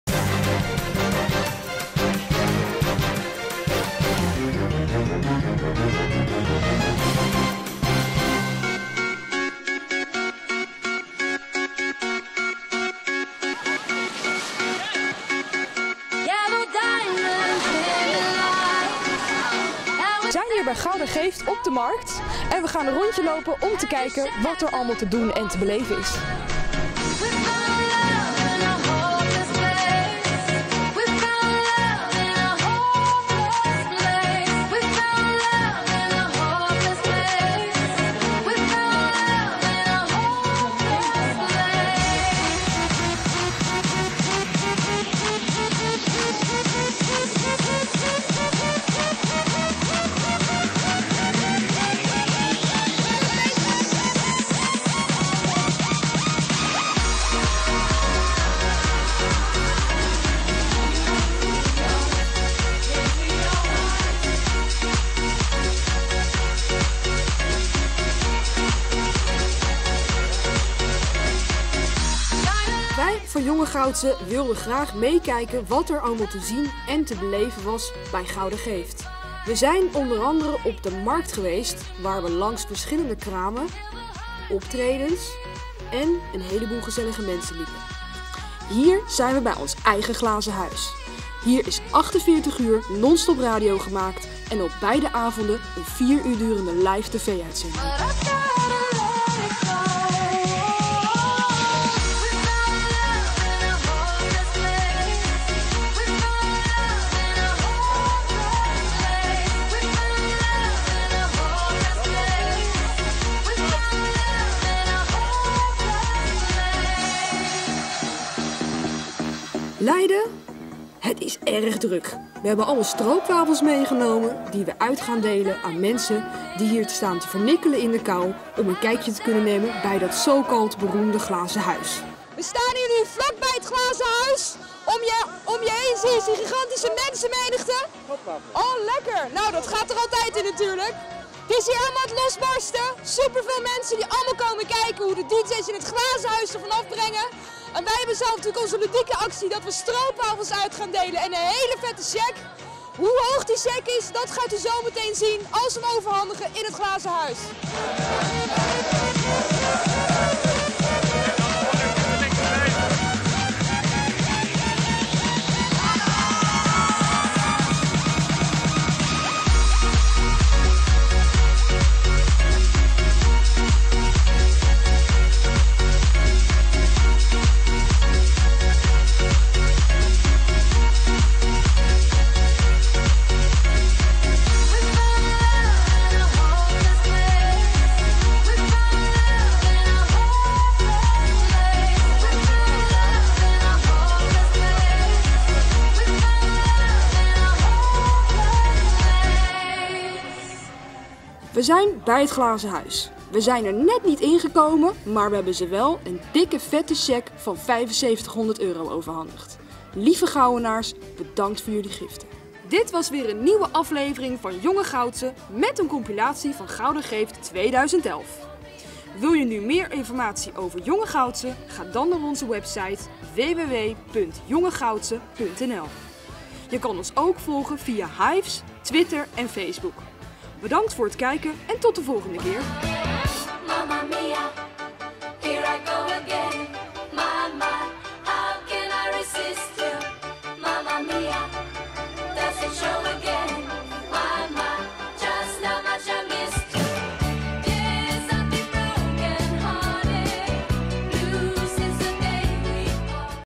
Jonge Goudse Afl. 9 (Compilatie Gouda geeft 2011)
Het team van Jonge Goudse maakte een compilatie van Gouda Geeft 2011.